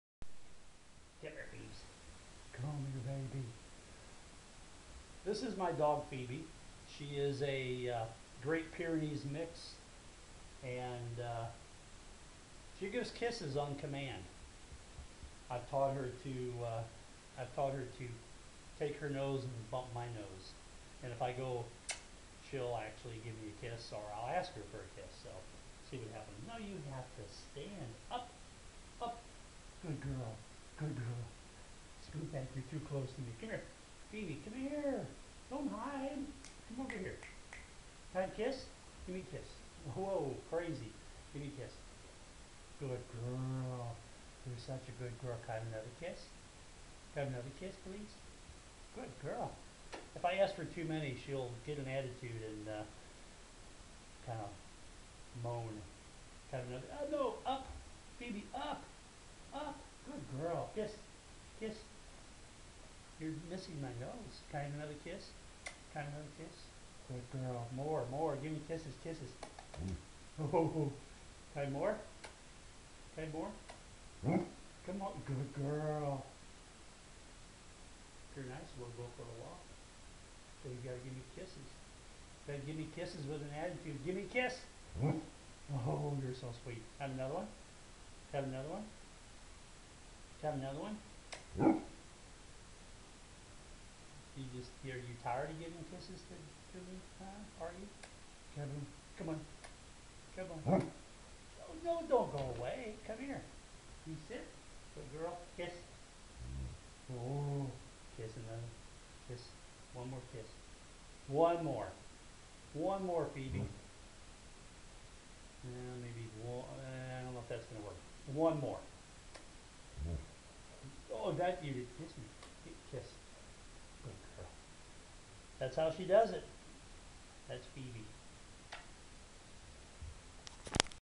Dog kisses eskimo style
My dog Phoebe, a 70lb Great Pyrenees mix gives kisses eskimo style